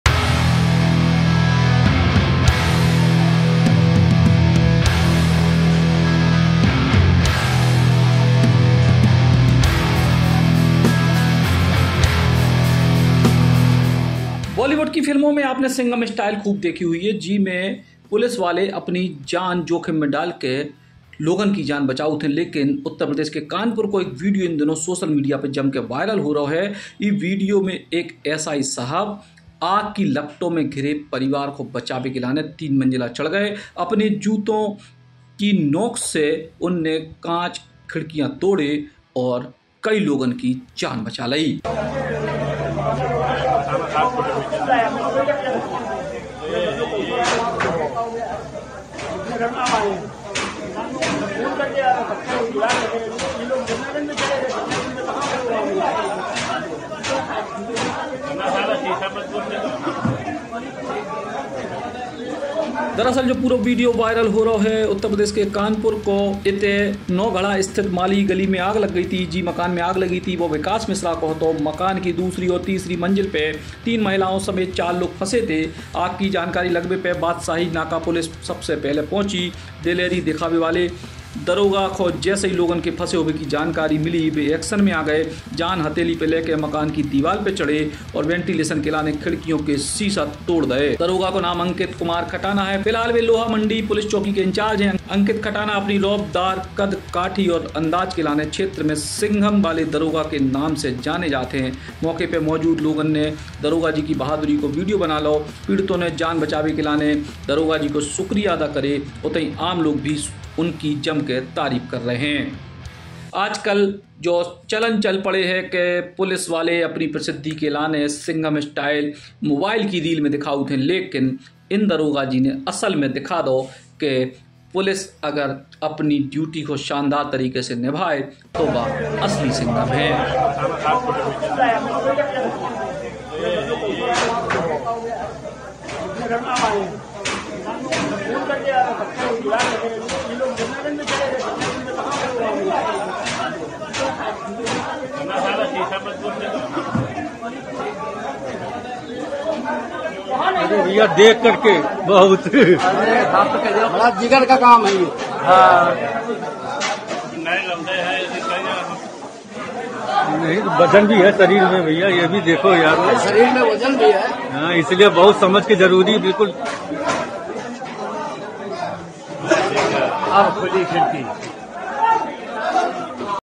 वीडियो वायरल - एक दरोगा बने असली सिंघम, कई लोगों की बचाई जान
BUNDELI BAUCHAAR

Suggested Pages :-